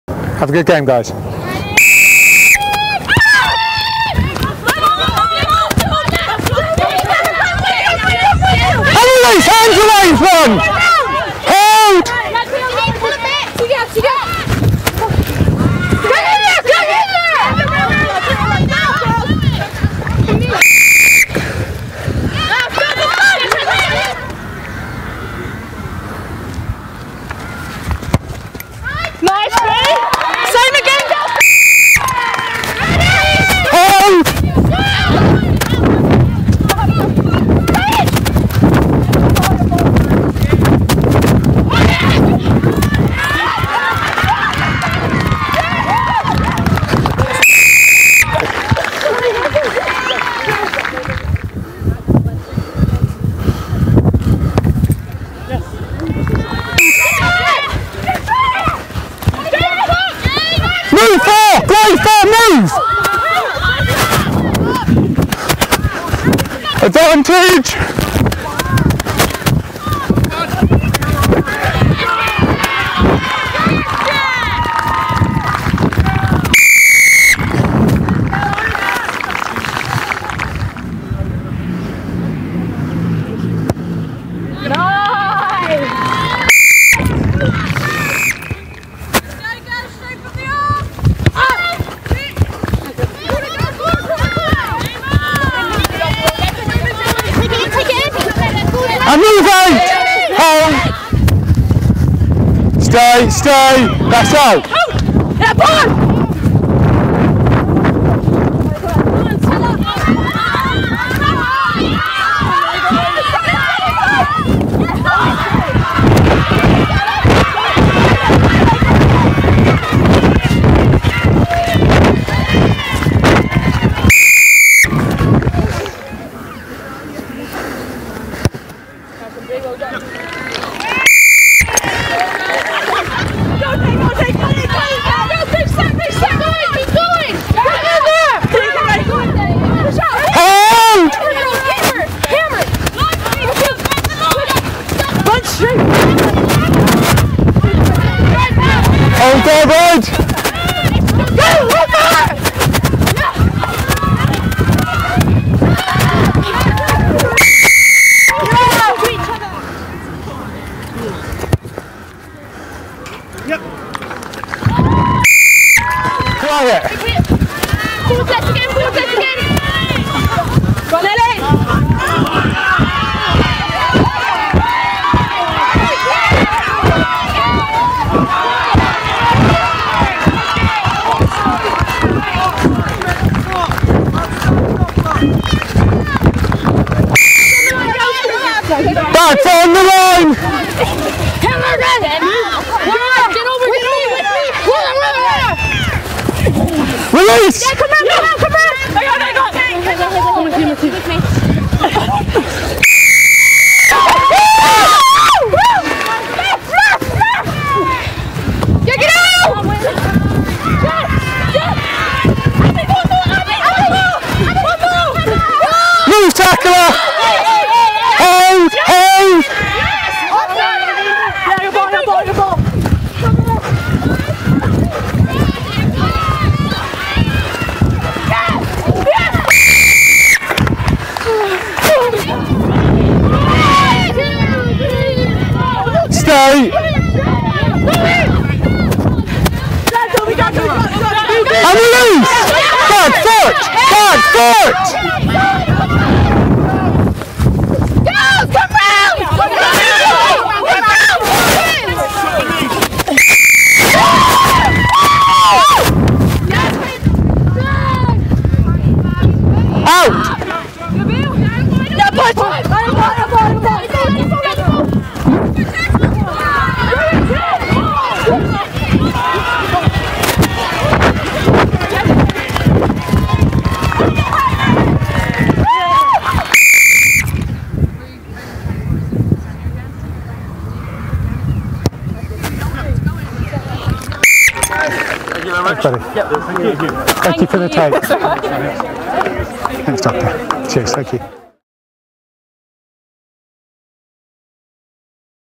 Canada U18 vs. England Talent Development Group — Game 1 Highlights
Canada's women's U18 team opened its tour of England with a 41-17 defeat against EnglandTalent Development Group Tuesday. Canada got tries from Taylor Black, Kendra Roberts and Sophie de Goede, who also had the lone conversion.